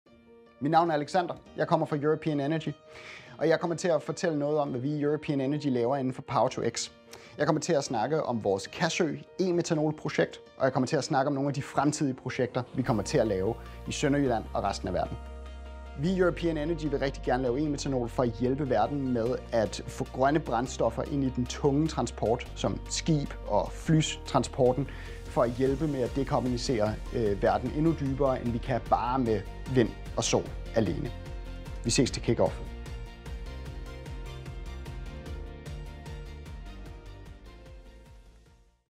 Bureau Veritas Kick Off 2024 | Project Associate, European Energy A/S
Verdens største e-metanolfabrik ligger i Kassø nær Åbenrå, og når den når en fuld kapacitet, vil den kunne producere 42.000 ton om året. På Bureau Veritas Kick Off 2024 vil du få et indblik i European Energy A/S, når Project Associate, Alexander Remuert, fortæller mere om projektets tilblivelse og visioner for Power-to-X som en del af fremtidens energiløsning.